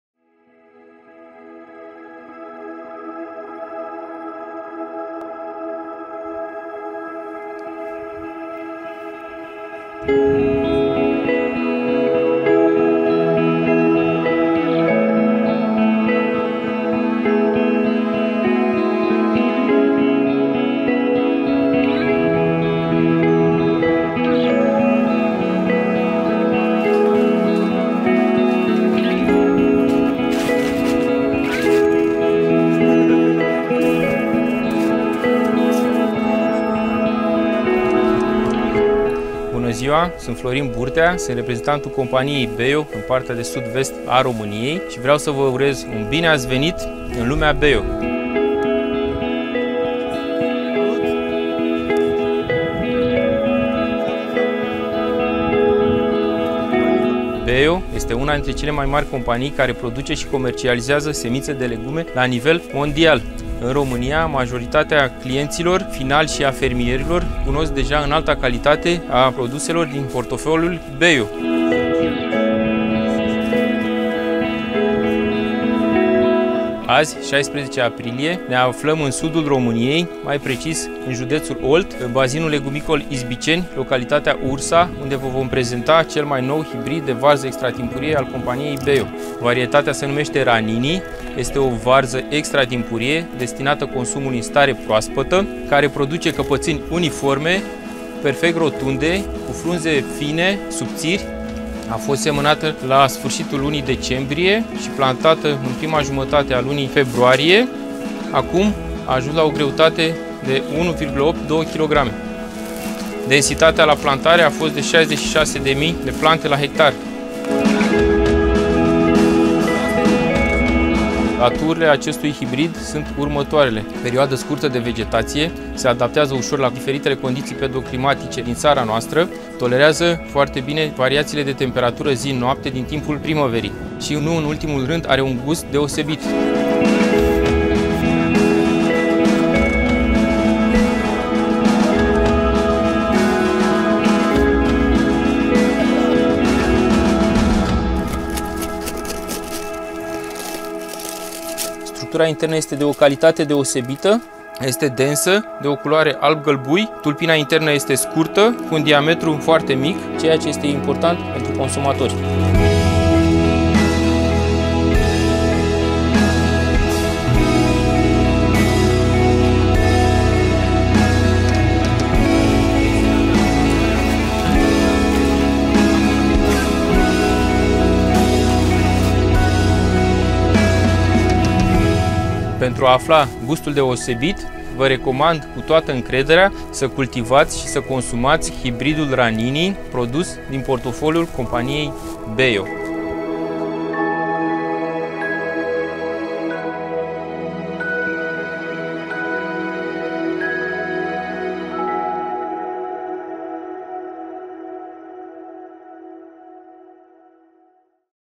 Ranini F1 - cel mai nou hibrid de varză extratimpurie al Bejo
Ranini F1 - hibrid de varză albă extra timpuriu și productiv.
Maturitatea după plantare (zile): 52
Formează căpățâni uniforme, rotunde, dense, cu o tulpină internă scurtă al cărei diametru este foarte mic. Frunzele sunt fine și subțiri.
Greutatea medie (kg): 1.8 – 2.0
Ranini se caracterizează printr-o perioadă scurtă de vegetație, se adapteză ușor diferitelor condiții pedoclimatice din țara noastră,
tolerează foarte bine temperaturile scăzute de la începutul primăverii și nu în ultimul rând, are un gust deosebit.

Recomandat pentru cultura în spații protejate
Densitatea folosită:  66.600 plante/ha
Întrebuințare: consum în stare proaspătă